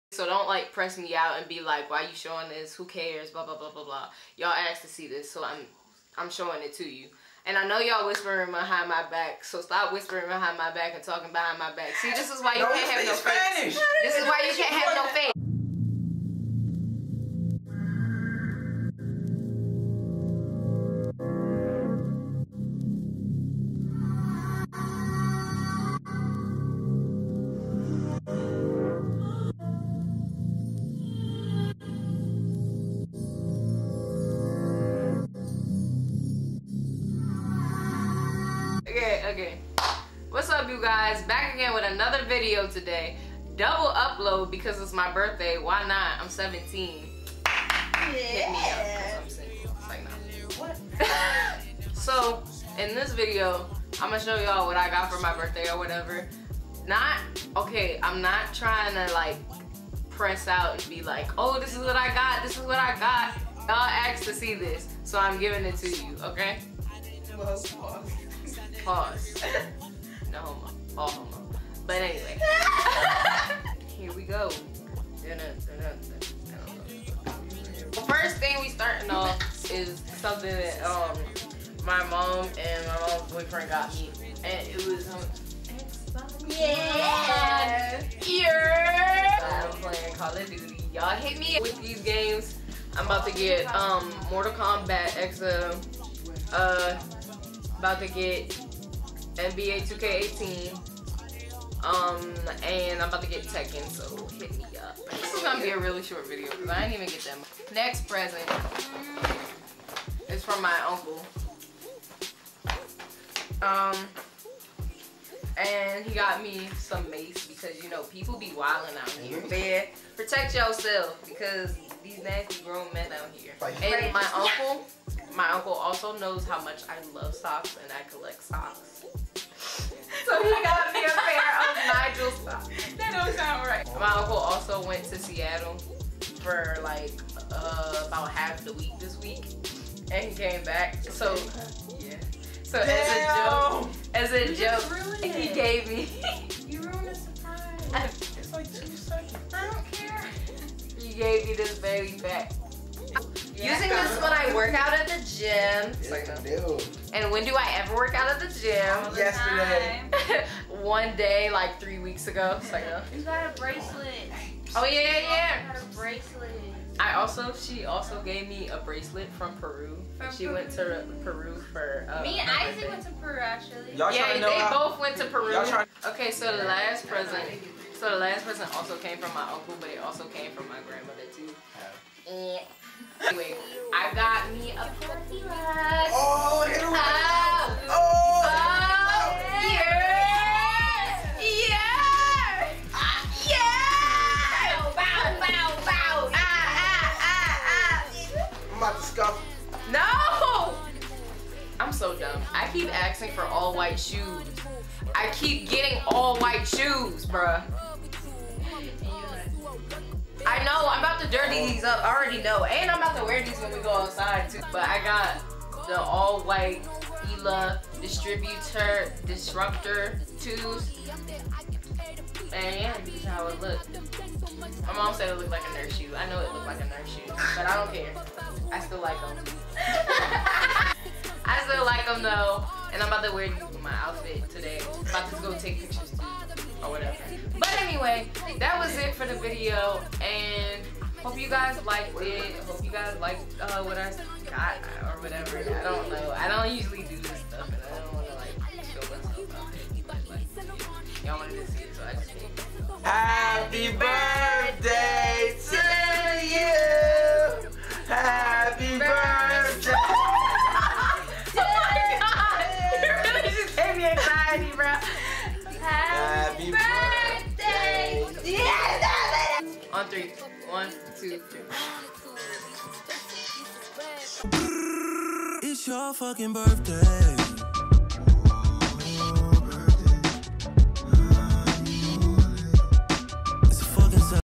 17th Birthday Haul 🎂💞🎉 // Dreamer Drea
Did someone say "double upload"? Yes, I'm back with another video but you guys wanted to see this. This video is not to make anyone feel bad or to show off. I'm not privileged or better than anyone else. This video is simply to show what I got because most of you requested to see it.

Go follow social medias too!!!